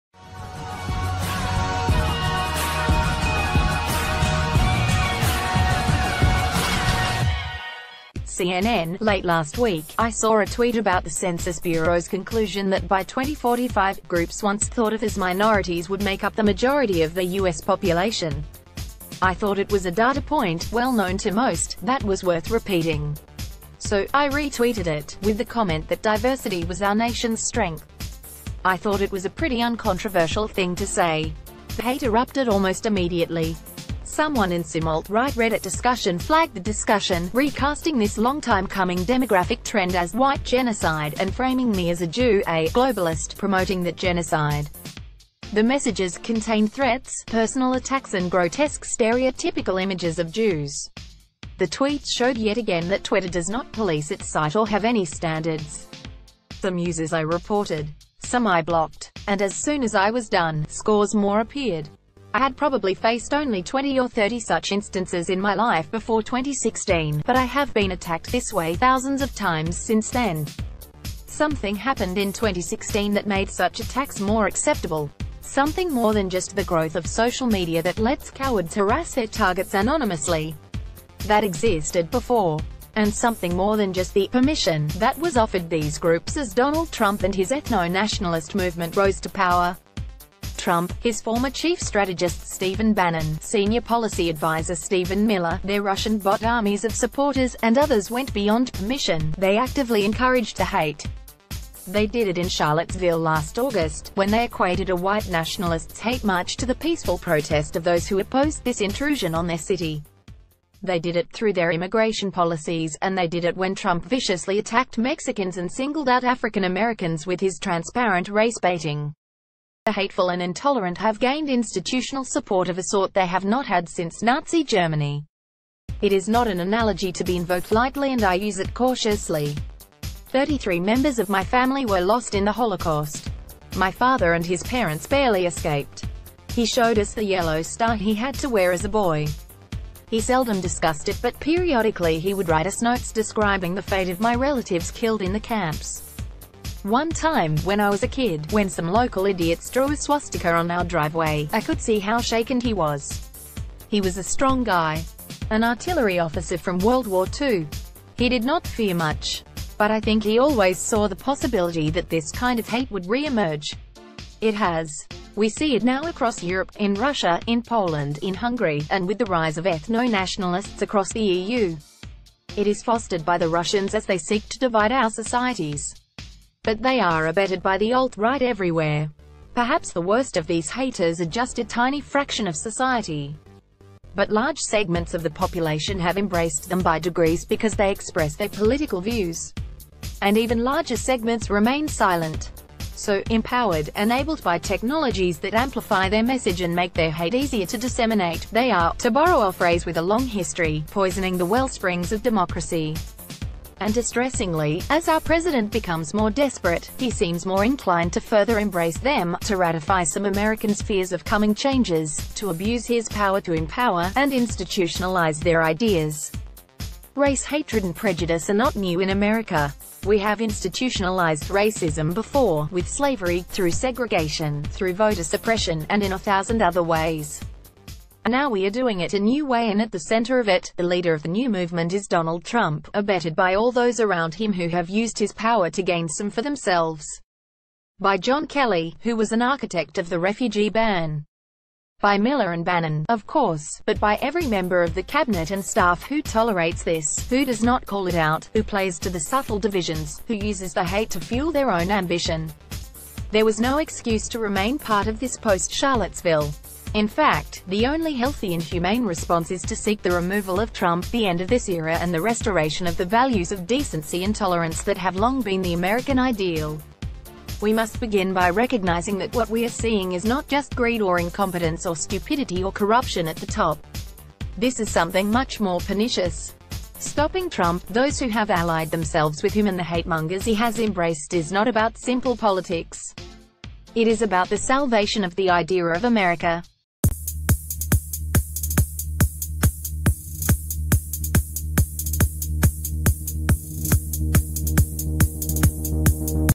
I tweeted about diversity and got hammered by haters | by News People Today
(CNN)Late last week, I saw a tweet about the Census Bureau's conclusion that by 2045, groups once th...